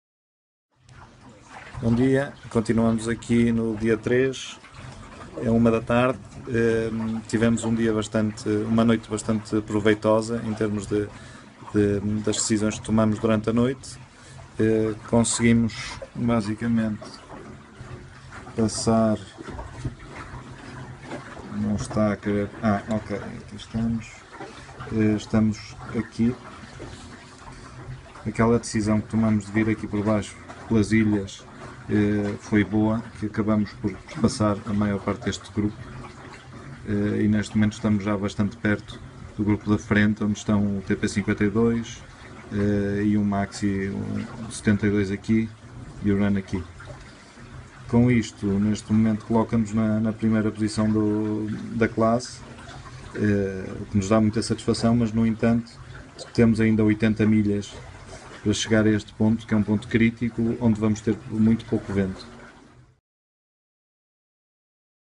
O Kuka-Light quase sem vento!
Com quase toda a frota parada, o Kuka-Light tirou partido de uma rota mais a sul, e segue na 10ª posição em tempo real!